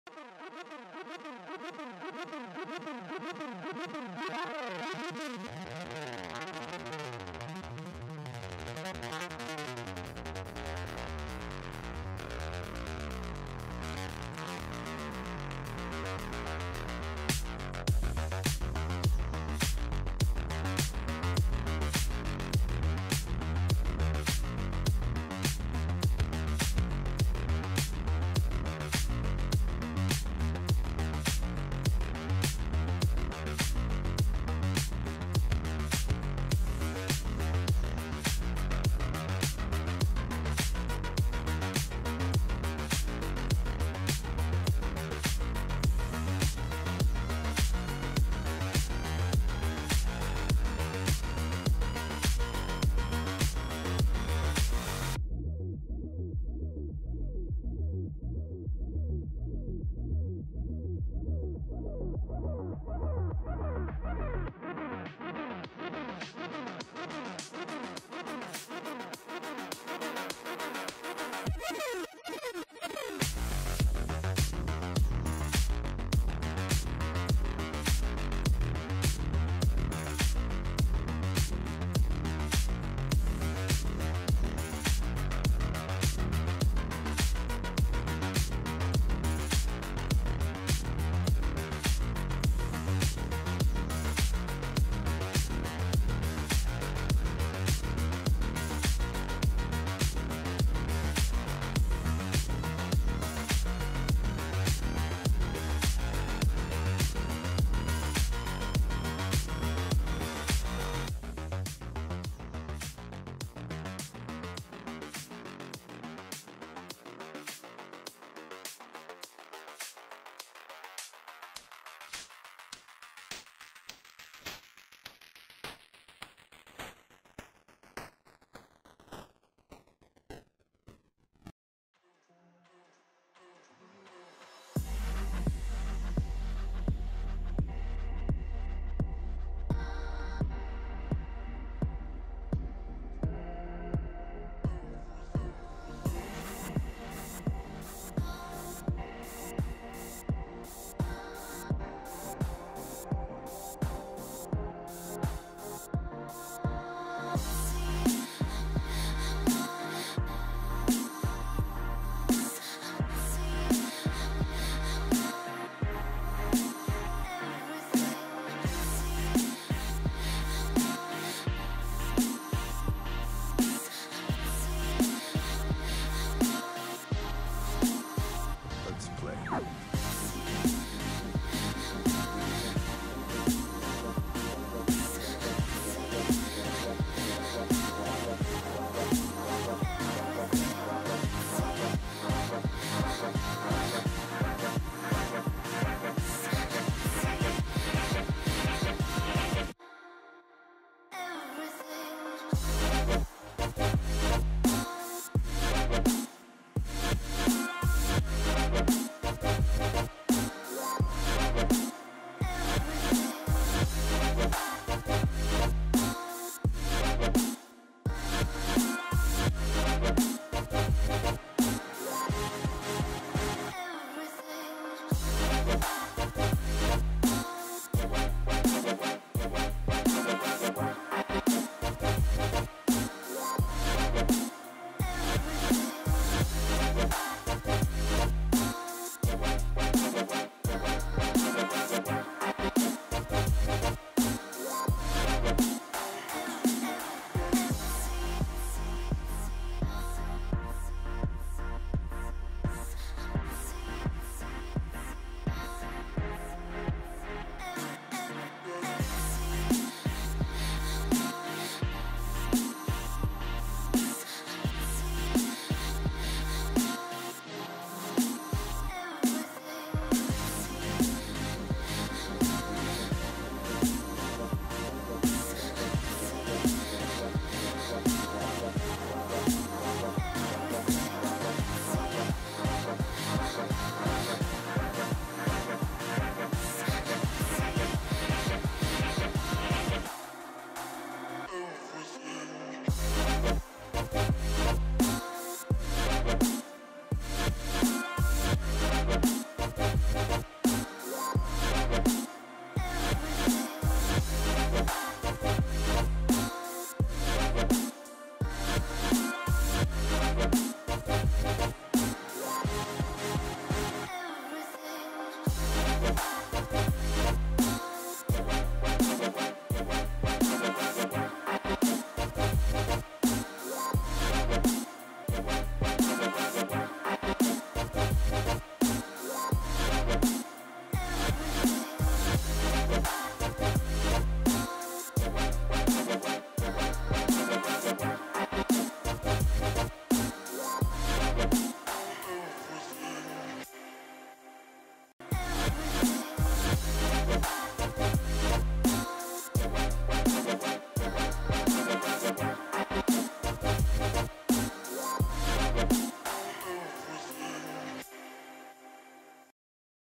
Knitted Caddis in 6 minutes How to Knit a Caddis Fly Tying Video
Hello, you are on the Inventor channel and in this video I will show the process of knitting the most catchy fly (nymph)
You will see the process of creating a fly step by step
How to Knit a Caddis Step By Step